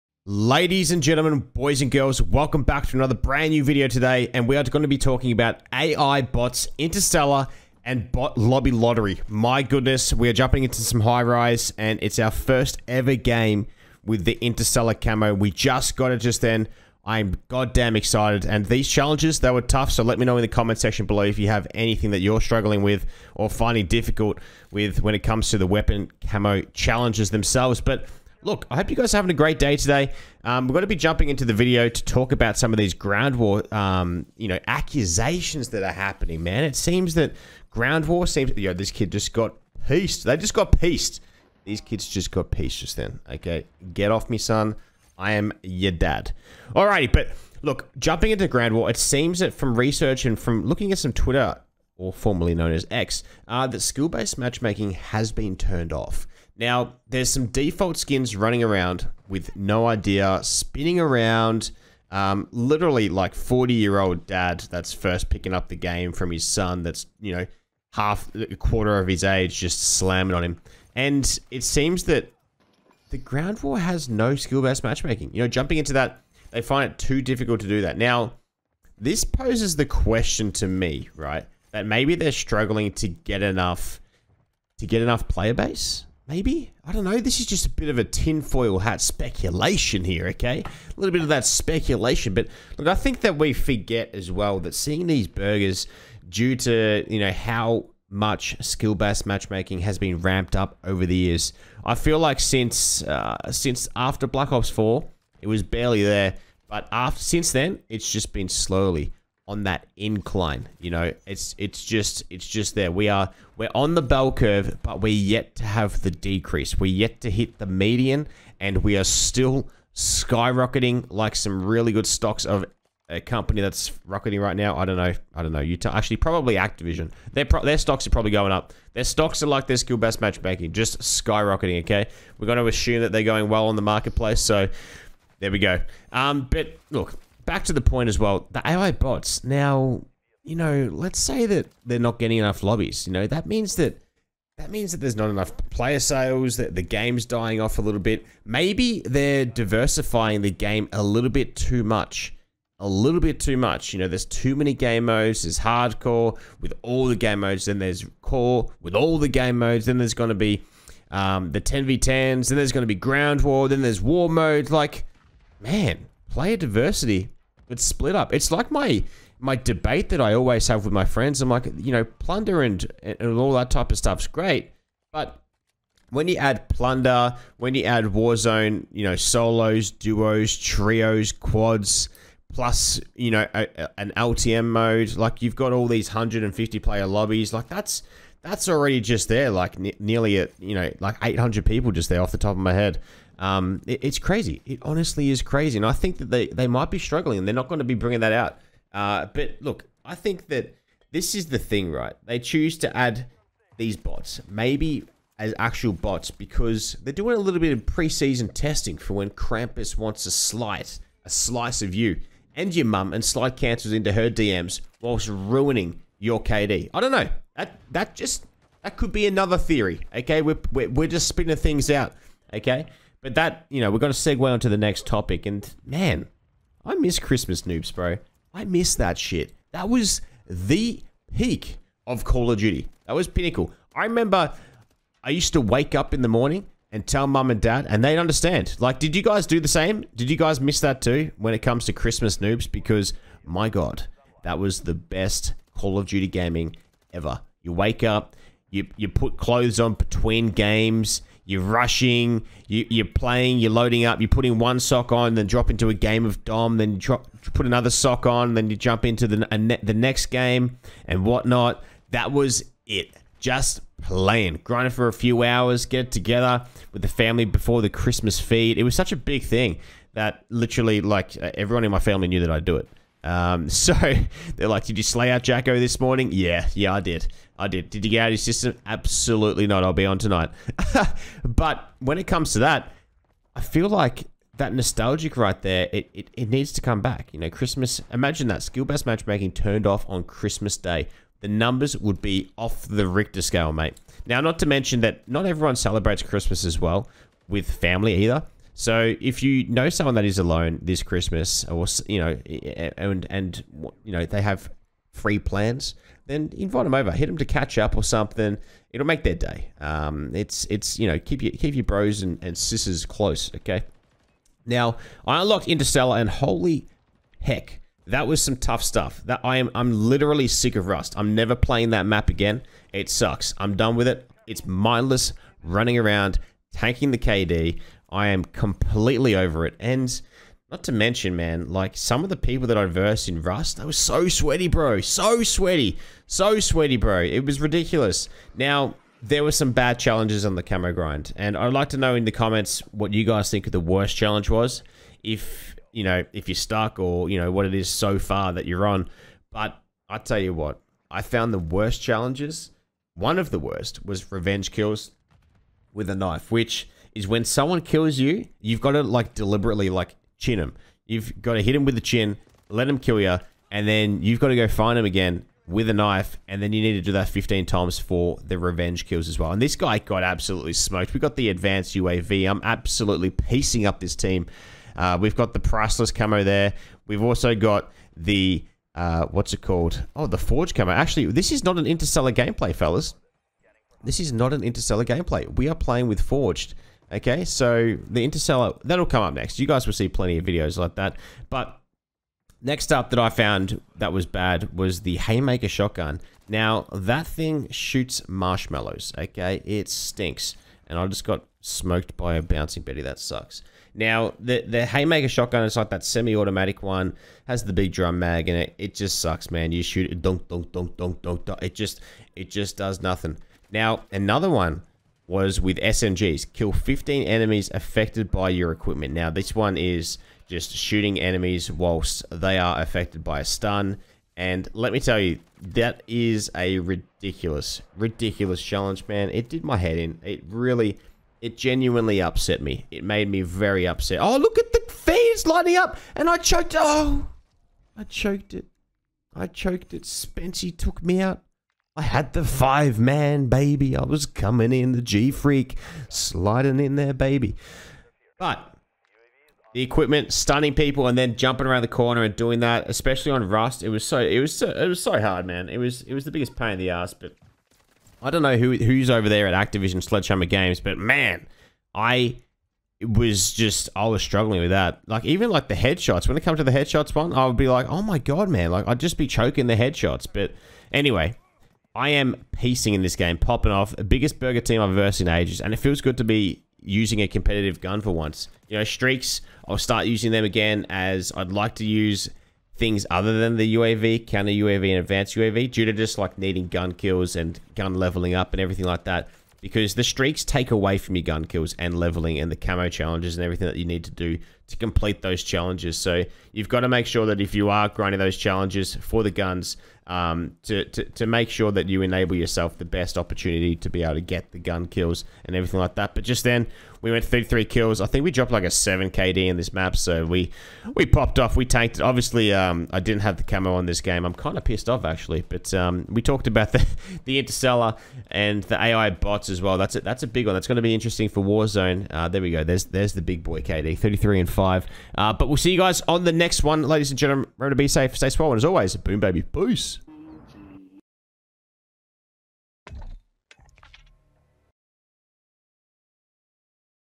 A.I. Bot Lobbies, Interstellar Grind & Christmas Noobs
Embark on a gaming odyssey with our latest video! Dive into AI Bot Lobbies, an Interstellar Grind, and a festive hunt for Christmas Noobs. Uncover the strategies to conquer these cosmic challenges and level up your gameplay. Don't miss the action-packed excitement—watch now!